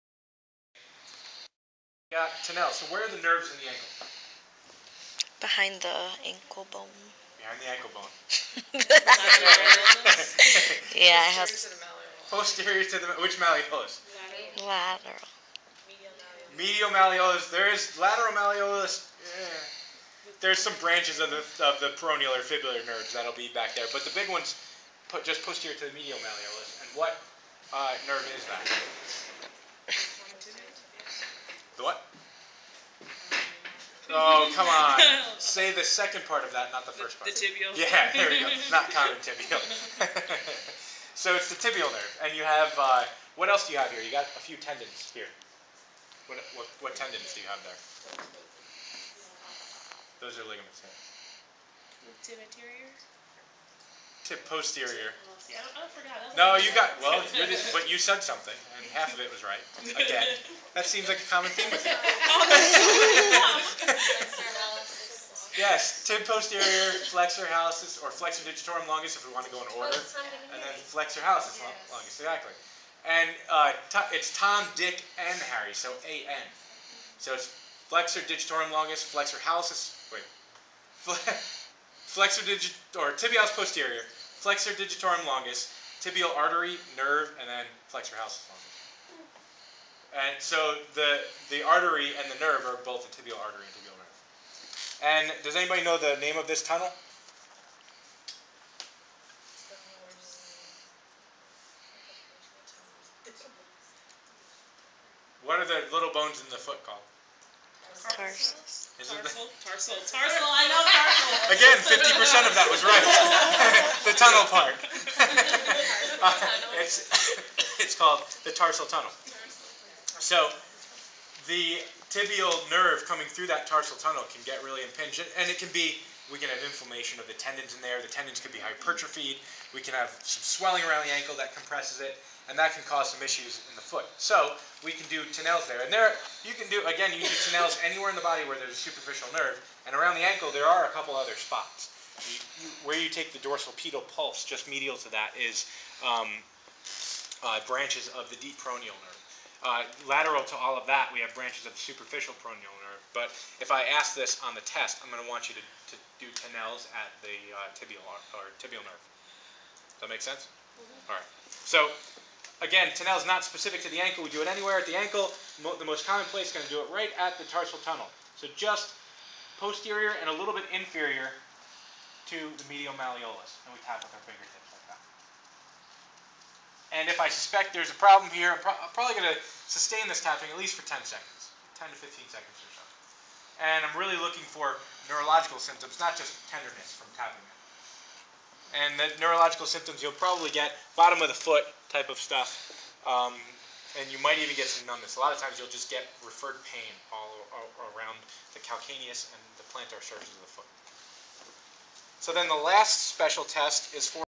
Tarsal Tunnel Syndrome and the Tinel's Test
How to test for Tarsal Tunnel Syndrome using the Tinel's sign. When tapping the posterior tibial nerve in the tarsal tunnel, a distally radiating sensation will occur if the nerve is pathologically compressed at this location.

This was filmed on a digital camera in 2012 for lower body orthopedic testing for medical students. Please consider this before posting comments about the video and audio quality.